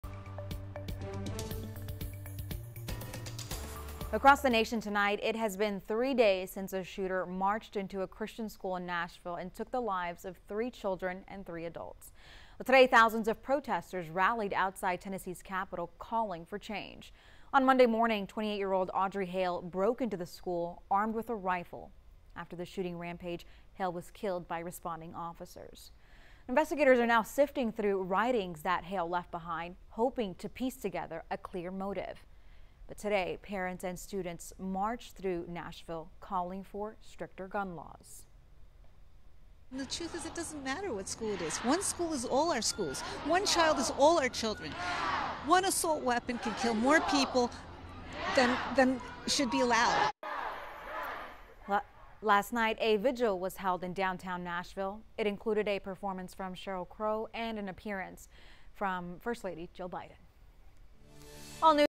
'Save our children!': Nashville protesters rally at capitol to demand gun control
Most protesters were removed from the gallery after some began yelling down at the lawmakers, “Children are dead!”